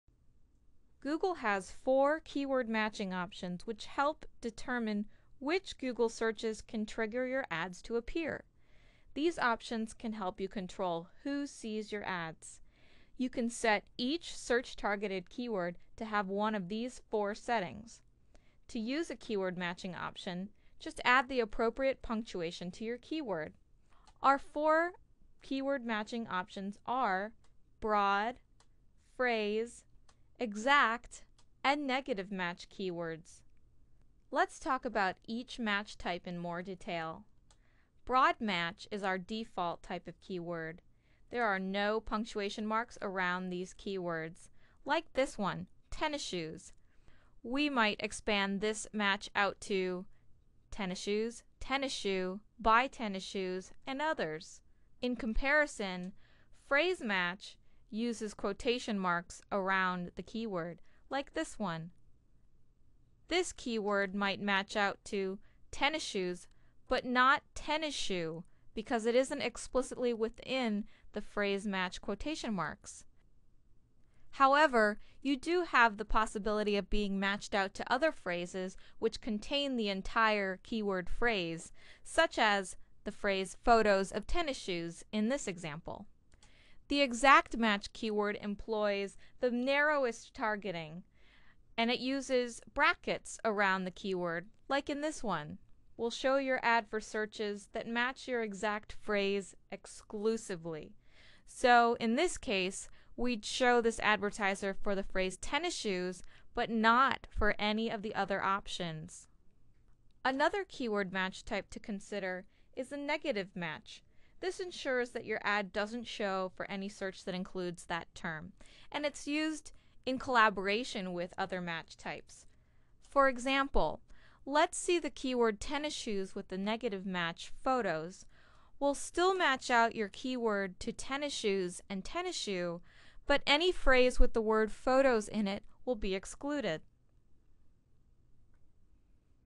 What are the keyword matching options?
Learn about the 4 types of keyword matching options: broad, phrase, exact, and negative match.